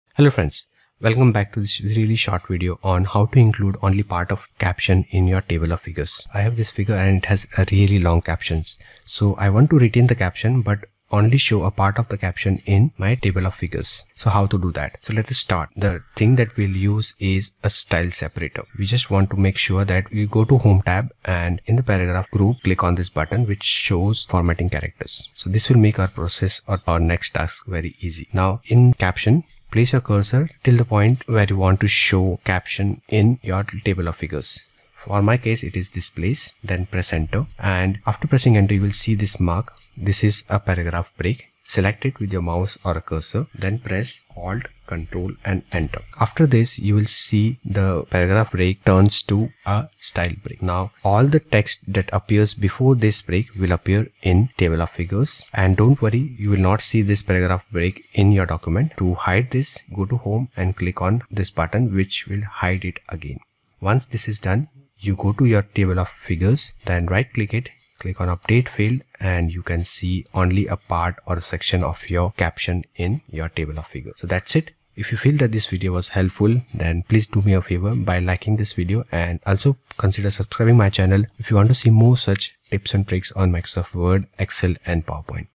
[✅Solved] How to Include Only Specific Parts of Figures in the Table of Figures in MS Word
Discover how to include only specific parts of a figure or caption in the Table of Figures in Microsoft Word. This step-by-step guide shows you how to customize captions, manage figure lists, and create a professional document layout. Perfect for reports, theses, and academic writing, this video will simplify your workflow and improve your document's structure.

Searches often focus on customizing and excluding captions, making these keywords more relevant for your audience. For detailed examples, you can refer to the Microsoft Support guide on Table of Figures